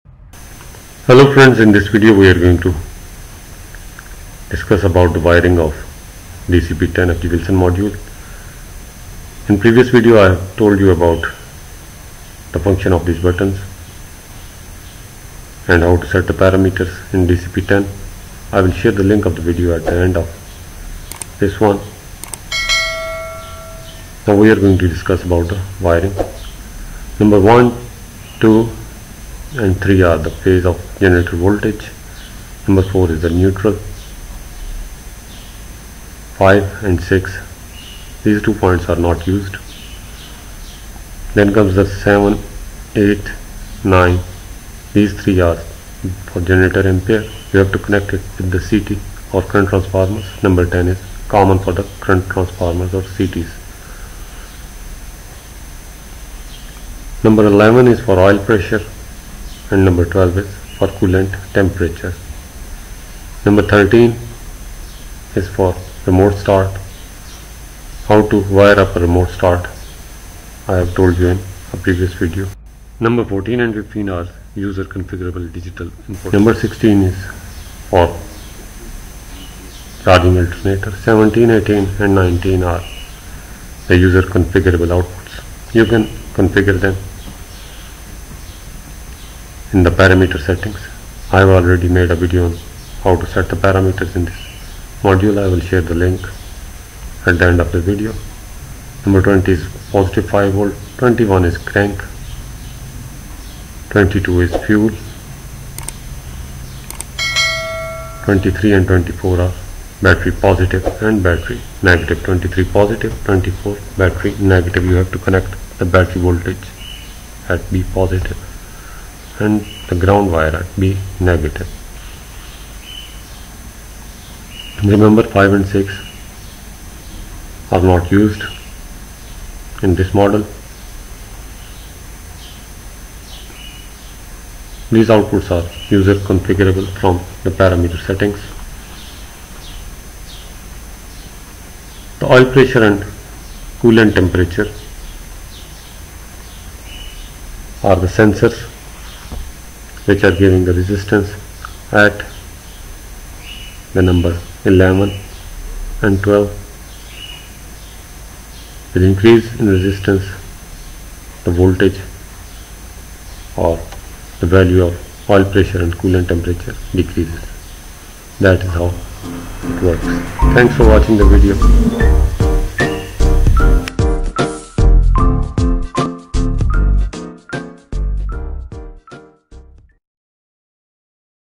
Dcp-10 Controller wiring learn the process in 3 minutes watch complete video.
The DCP-10 is an Automatic Controller for the generator. When running in "AUTO" mode, it starts the Genset after receiving the remote start signal and failure automatically stops the Genset. The generator's controlling procedure and protection parameters can be modified, which fully meets Genset's requirements of automatic start, stop control, and basic protection. The module displays fault conditions, operational status, and related metering data on the panel LCD.LCD has a backlight function so that the operator can read running parameters clearly even in the shadow. The controller has 2 modes: AUTO and MANUAL. Either can be chosen through the panel push button. Measures and displays the generator's output voltage, current, oil pressure, coolant temperature, frequency, DC source voltage, etc. True RMS measure of voltage and current, which ensures the data is more accurate. Control the close/open of the generator output switch. Equipped with a built-in communication interface to configure parameters by PC. All connections of the controller are by secure plug and socket, for ease and convenience to connect, move, maintain, and replace the device.